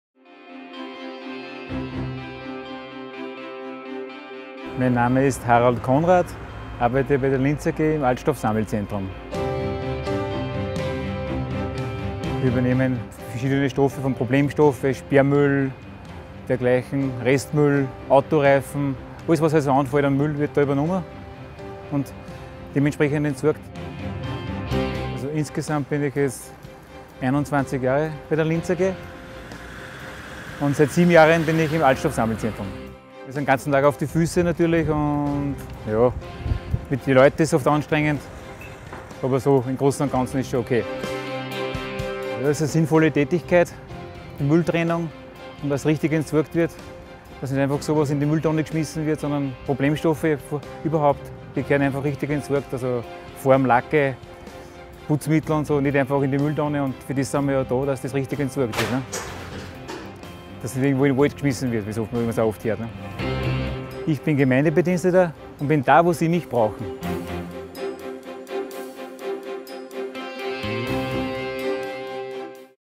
Recyclinghof Linz - Wir sind da, wo Sie uns brauchen.
24 Stunden pro Tag, 365 Tage im Jahr sorgen die Gemeindebediensteten in 2.357 Städten und Gemeinden dafür, dass die Menschen in diesem Land ihr Leben leben können -- und sich keine Gedanken darüber zu machen brauchen, ob ihr Müll abtransportiert wird, sauberes Wasser aus der Leitung kommt oder ob sie im Notfall in einem Spital behandelt werden. In vielen Ländern der Welt ist das nicht so selbstverständlich wie in Österreich.